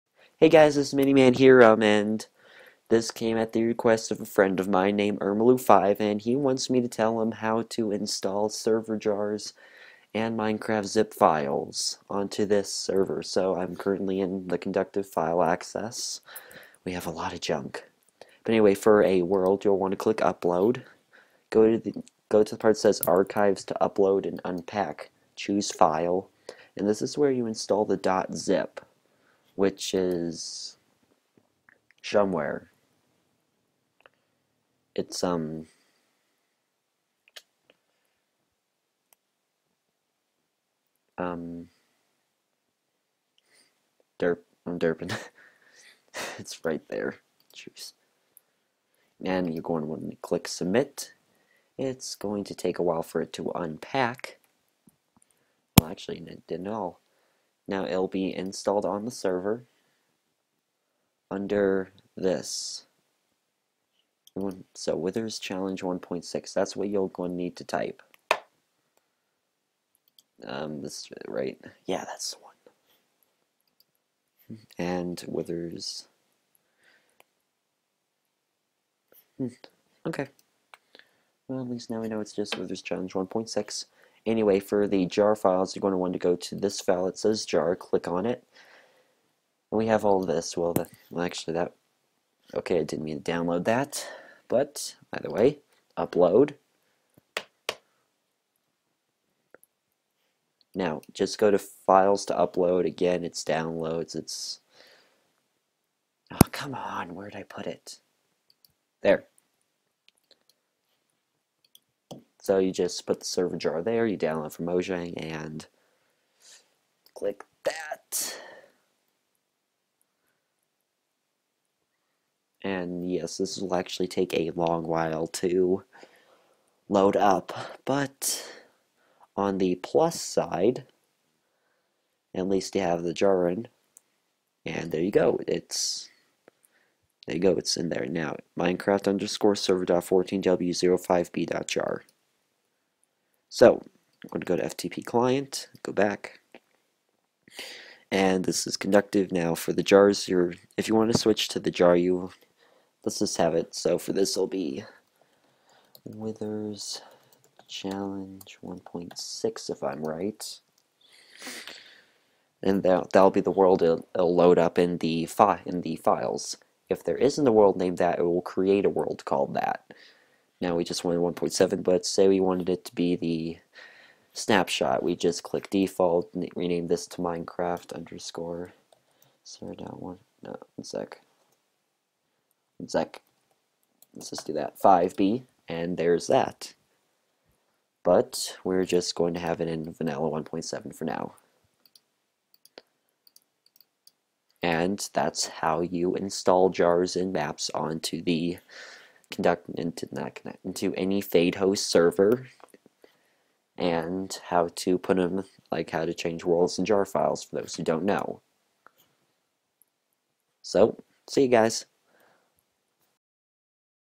FadeHost Tutorial
Here's a simple tutorial on how to upload maps and jar files to FadeHost servers.